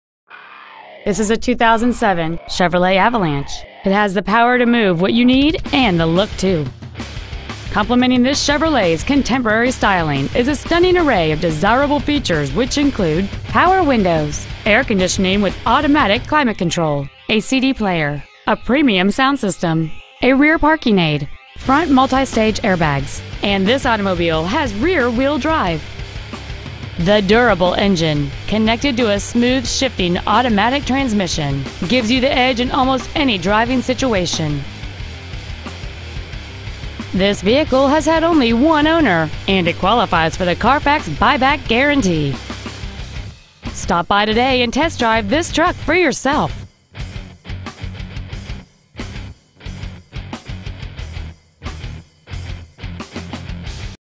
Used 2007 Chevrolet Avalanche Arlington TX 76017
Year: 2007
 Make: Chevrolet
 Model: Avalanche

 Trans.: Automatic
 Exterior: Gold Mist Metallic
 Miles: 17,777
 Interior: Tan